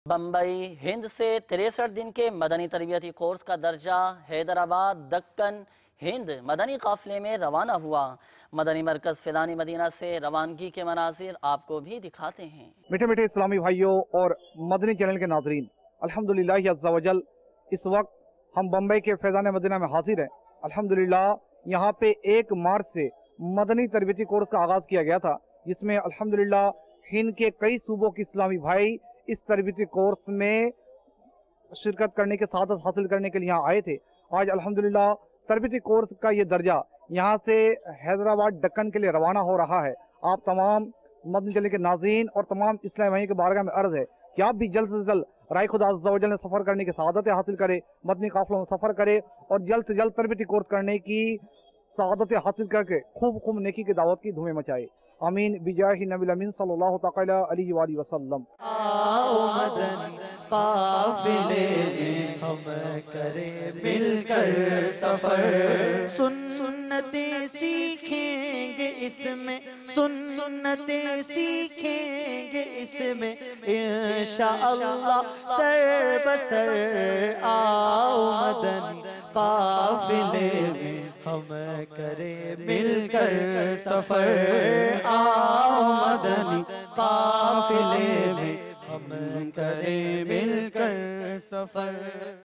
Madani News 31 March - Madani Pearls of Muballigh-e-Dawateislami during the Madani Tarbiyyati Course
This video  contains Islamic News Updates of Dawateislami in Urdu With English Subtitle for the Day of (News 31 March).